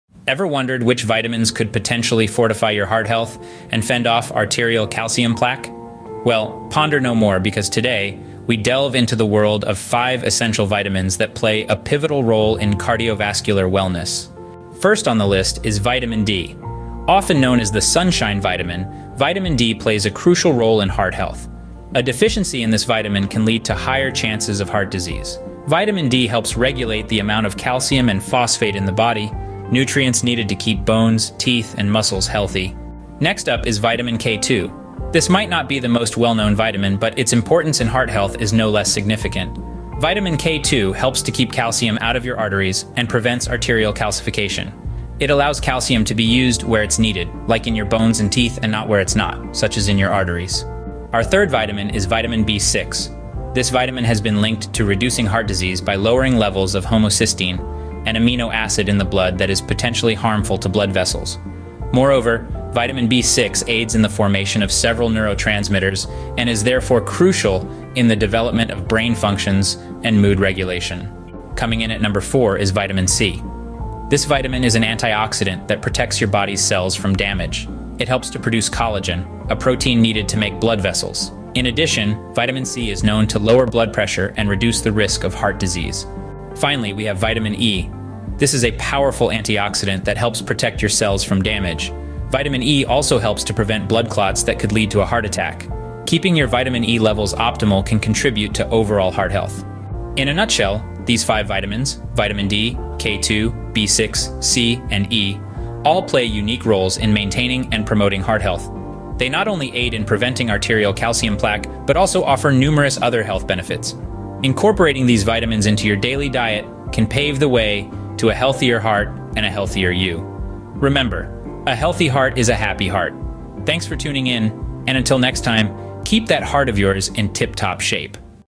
5 Key Vitamins to Boost Heart Health: Unlock Heart-Healthy Vitamins
Learn about 5 key vitamins that can help boost your heart health! In this video, we'll uncover the top vitamins that can benefit your heart and overall well-being. It's time to unlock heart-healthy vitamins and take control of your health.
In this video, we delve into the crucial role that 5 key vitamins play in enhancing heart health and warding off arterial calcium plaque. Join us as we explore the significance of these essential nutrients and how they contribute to a healthier heart. Understanding the impact of vitamins on cardiovascular health is vital for overall well-being.
OUTLINE:
00:00:00 Exploring the World of Heart-Healthy Vitamins
00:00:14 The Role of Vitamin D in Heart Health
00:00:32 Understanding the Importance of Vitamin K2
00:00:50 The Link Between Vitamin B6 and Heart Disease
00:01:11 The Antioxidant Power of Vitamin C
00:01:26 The Role of Vitamin E in Protecting Heart Health
00:01:39 The Impact of Five Essential Vitamins on Heart Health